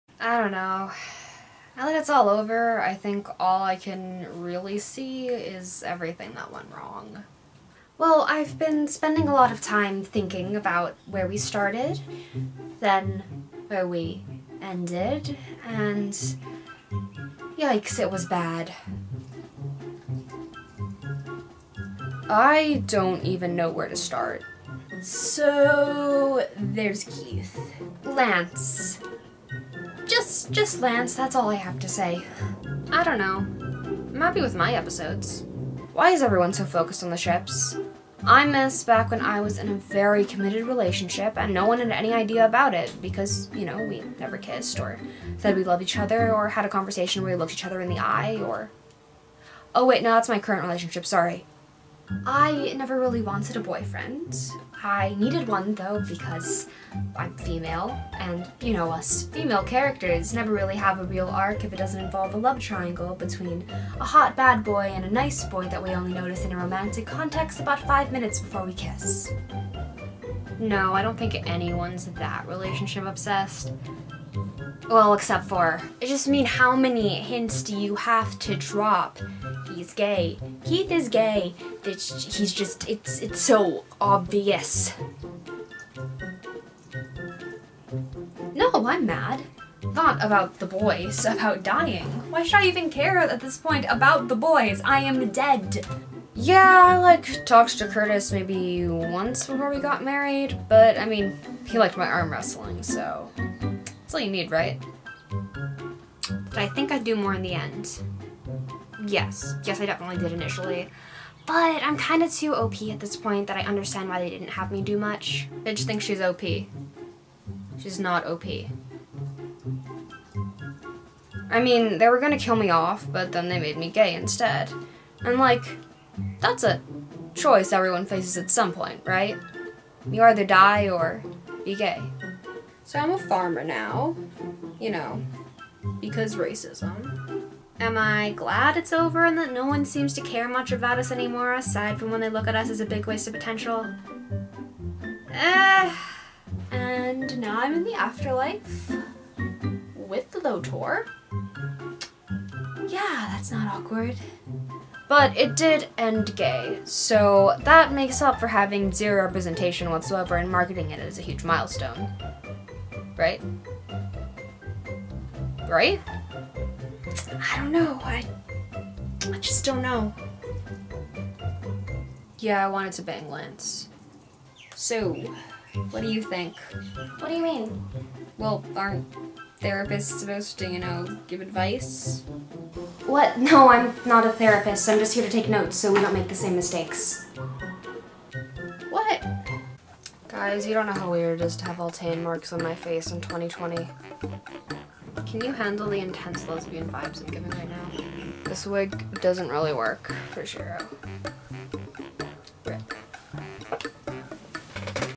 Voltron Therapy
How are the Voltron characters coping a year later?